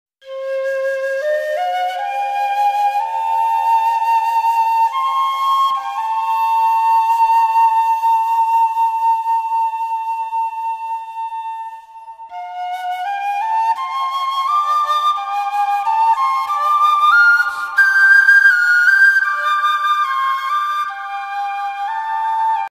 Papillons en liberté / Butterflies Go Free [ Jardin Botanique de Montréal ]
Papillons en liberté au jardin botanique de Montréal.
Butterflies go free at the Jardin botanique de Montréal.

This is an Idea leuconoe butterfly.